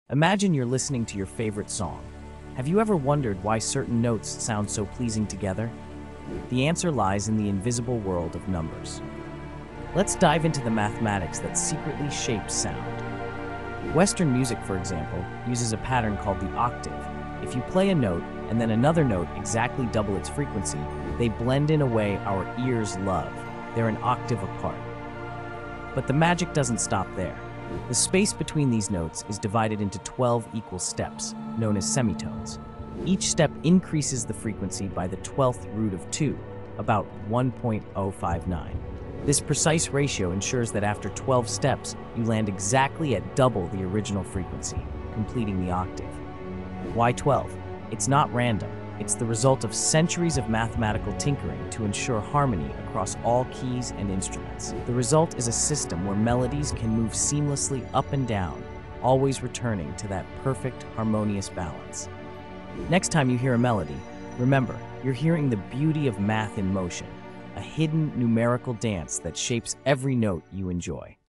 Unlocking the Math Hidden in Musical Scales
Discover the mind-blowing math that shapes the sound of music! Did you know numbers define how we hear harmony? Dive in!